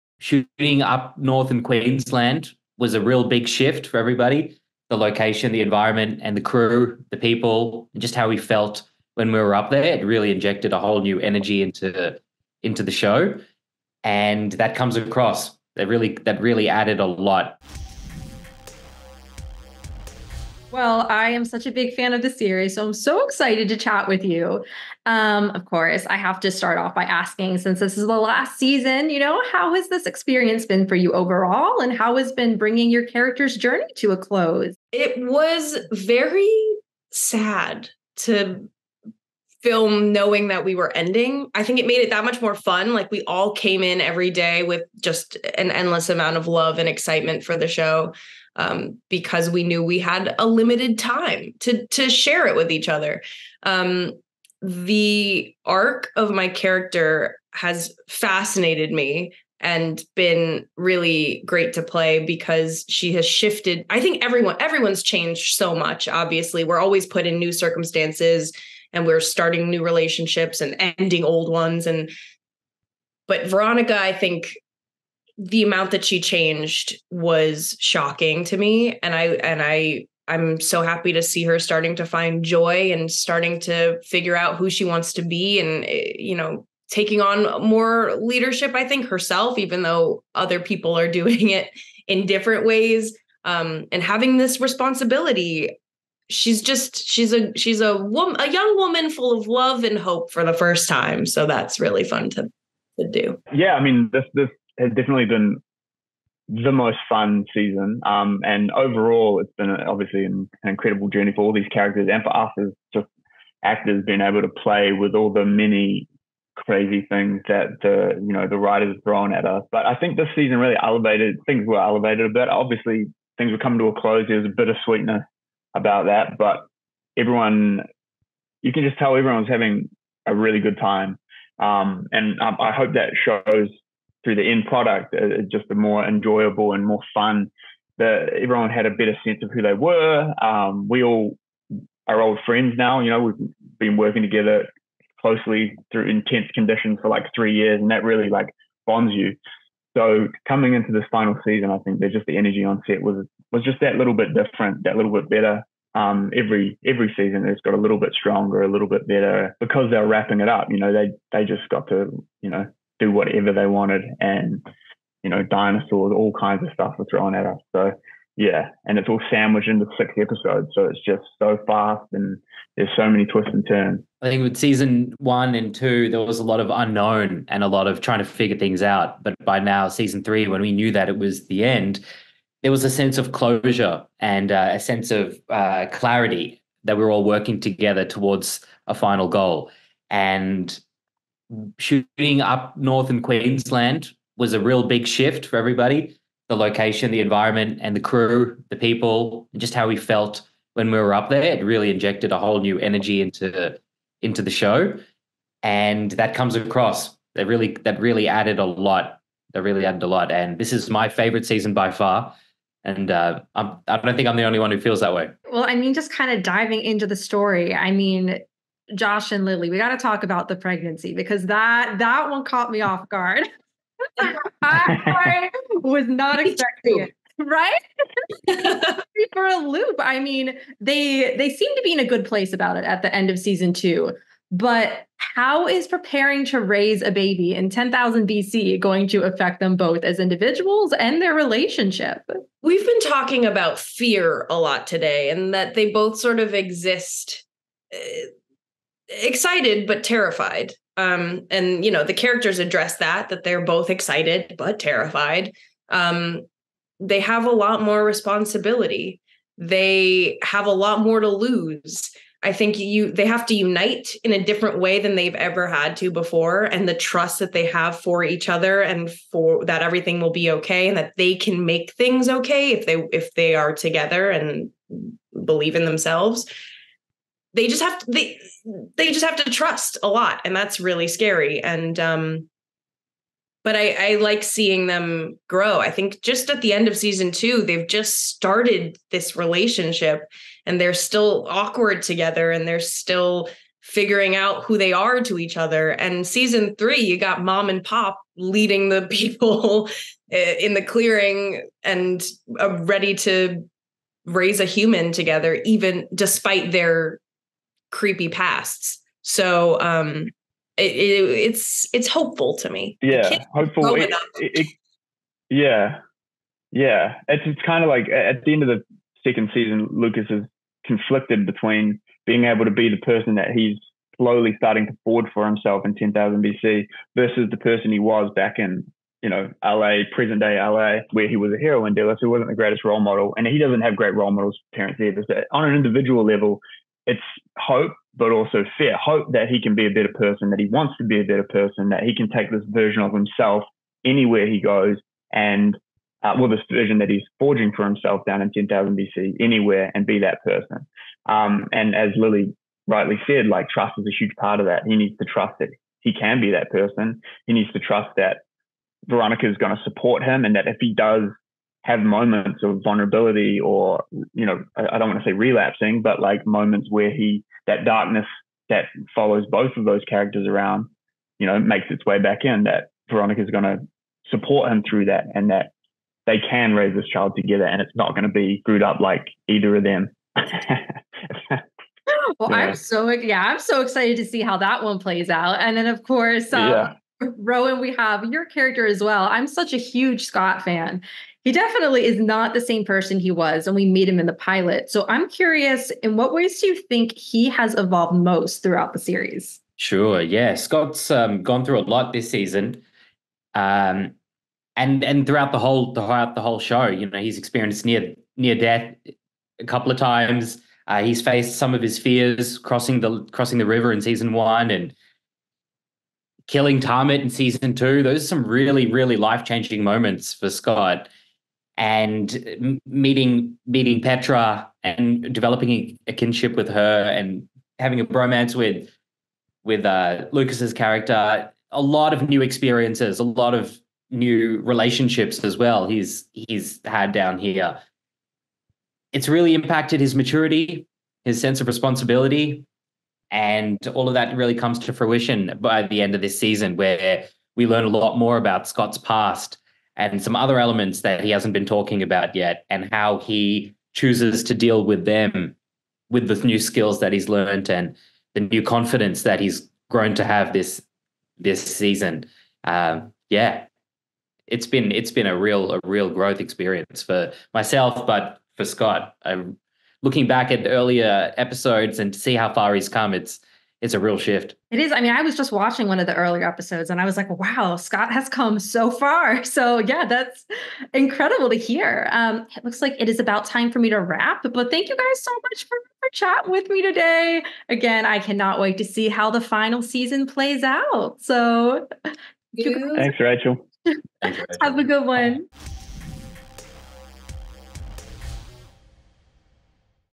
La Brea Interview: Lily Santiago, Josh McKenzie & Rohan Mirchandaney On Love And Life In 10,000 BC
Screen Rant interviews Lily Santiago, Josh McKenzie, and Rohan Mirchandaney about Veronica's pregnancy and romance with Lucas in La Brea season 3.

La Brea season 3 premieres Tuesday, January 9 at 9pm ET on NBC and is available to stream the next day on Peacock.